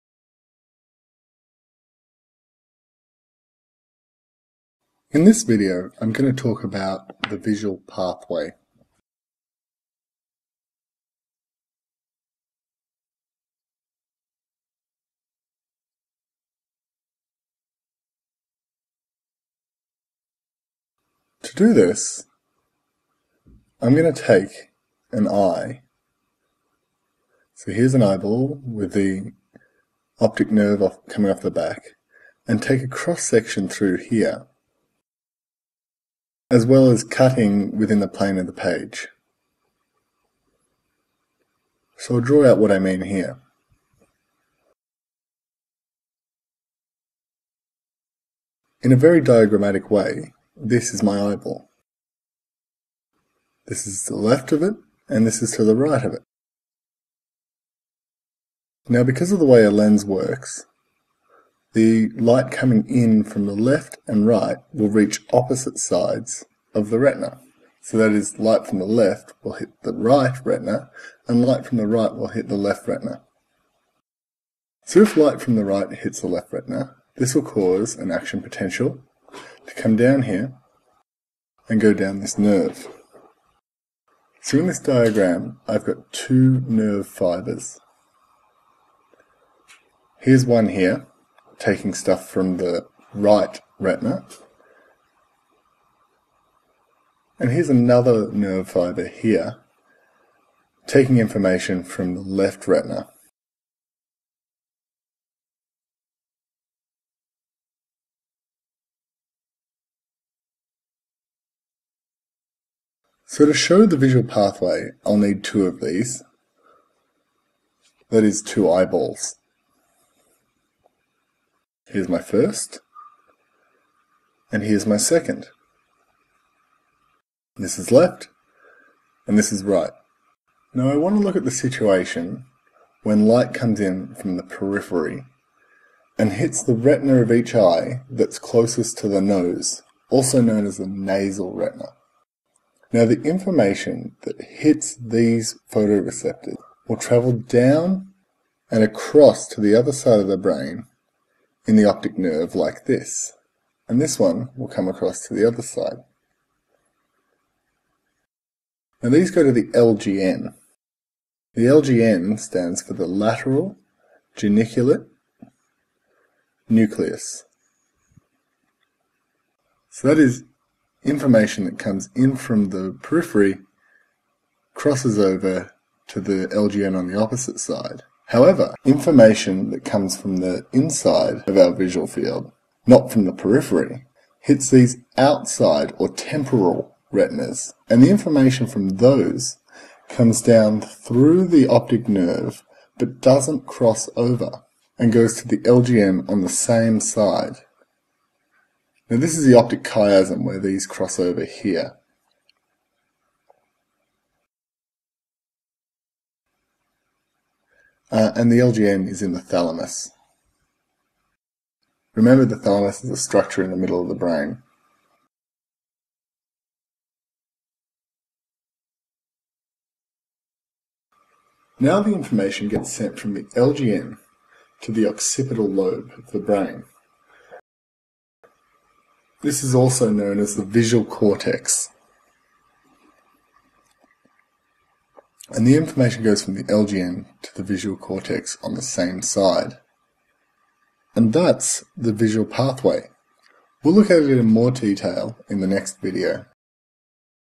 Visual Pathway (Vision 4 of 5)
- The fourth video in the Vision series. This tutorial explores the Visual pathway, from Retina, to LGN, to the Visual Cortex.  For more entirely FREE tutorials and accompanying PDFs